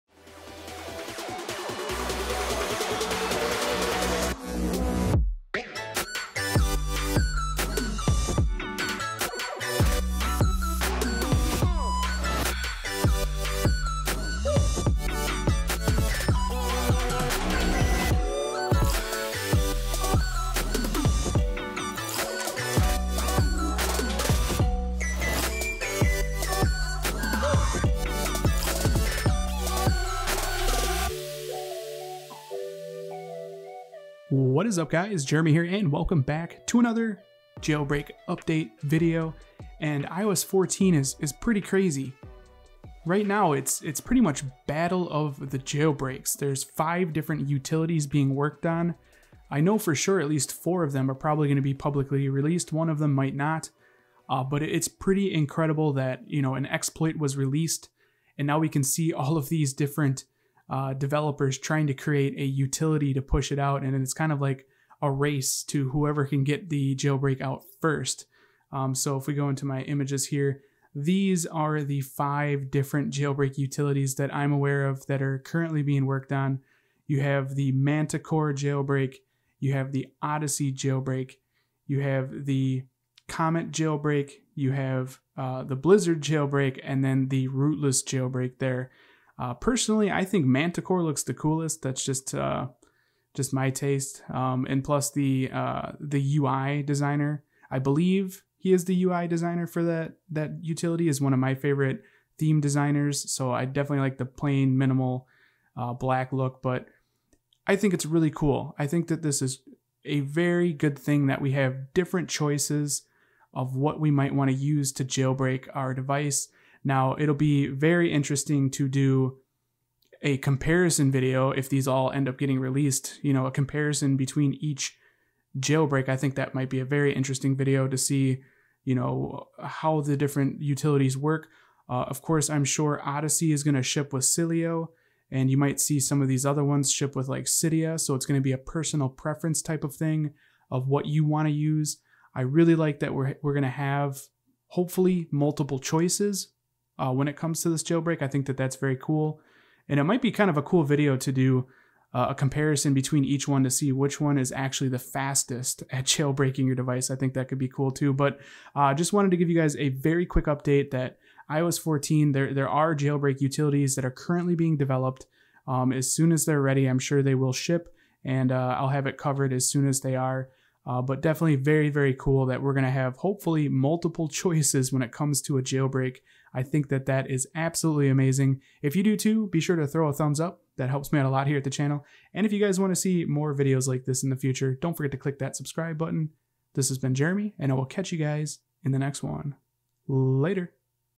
iOS 14 Battle Of The Jailbreaks! iOS 14.0-14.3 Odyssey / Manticore / Comet / Blizzard / RootlessJB
iOS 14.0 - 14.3 jailbreak is seeming to be a battle for the first. We have multiple developers creating multiple different jailbreak utilities. This is awesome, I think having choices is amazing and having the opportunity to choose what jailbreak you wanna use will be epic!

I TWEET TOO MUCH - FOLLOW

iHEAVEN DISCORD
COME CHILL & CHAT - BEST iOS SETUP SPOT

iDM REPO
ADD MY REPO IN CYDIA

DOPE APPS!